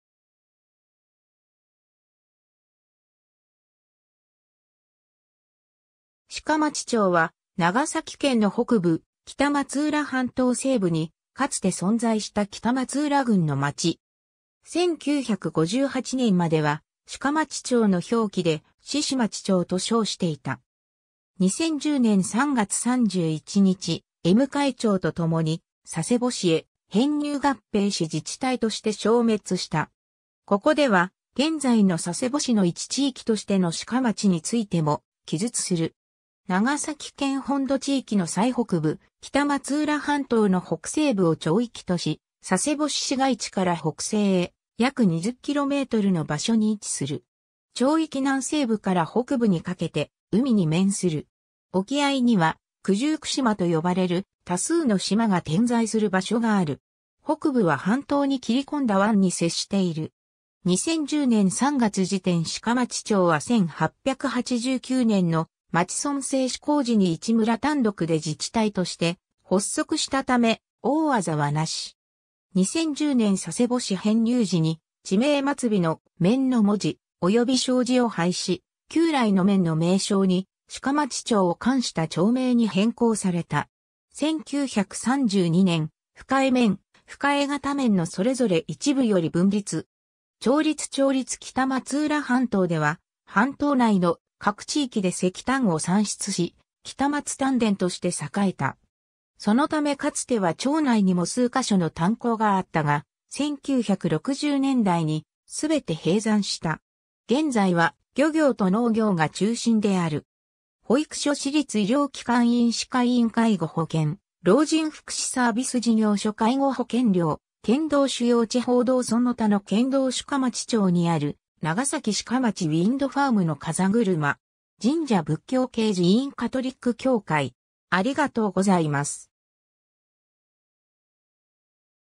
鹿町町
鹿町町（しかまちちょう）は、長崎県の北部、北松浦半島西部にかつて存在した北松浦郡の町。
1958年までは鹿町町の表記で「ししまちちょう」と称していた。
 2010年（平成22年）3月31日、江迎町とともに佐世保市へ編入合併し自治体として消滅した。
 ここでは、現在の佐世保市の一地域としての鹿町（しかまち）についても記述する。
 長崎県本土地域の最北部、北松浦半島の北西部を町域とし、佐世保市市街地から北西へ約20kmの場所に位置する。
町域南西部から北部にかけて海に面する。
沖合いには九十九島と呼ばれる多数の島が点在する場所がある。
北部は半島に切り込んだ湾（江迎湾）に接している。
 2010年（平成22年）3月時点 鹿町町は1889年（明治22年）の町村制施行時に1村単独（当時は鹿町村）で自治体として発足した為、大字は無し。
 2010年（平成22年）佐世保市編入時に地名末尾の「免」の文字、及び小字を廃し、旧来の免の名称に「鹿町町」を冠した町名に変更された。
 （例：北松浦郡鹿町町下歌ヶ浦免 → 佐世保市鹿町町下歌ヶ浦） ^ 1932年（昭和7年）、深江免・深江潟免のそれぞれ一部より分立。
 町立 町立 北松浦半島では半島内の各地域で石炭を産出し、北松炭田として栄えた。
そのためかつては町内にも数ヶ所の炭鉱があったが、1960年代にすべて閉山した。
 現在は漁業と農業が中心である。
 保育所 私立 医療機関 医院 歯科医院 介護保険・老人福祉 サービス事業所（医療系みなし指定除く） 介護保険料 県道 主要地方道 その他の県道 鹿町町にある長崎鹿町ウィンドファームの風車。
 神社 仏教系寺院 カトリック教会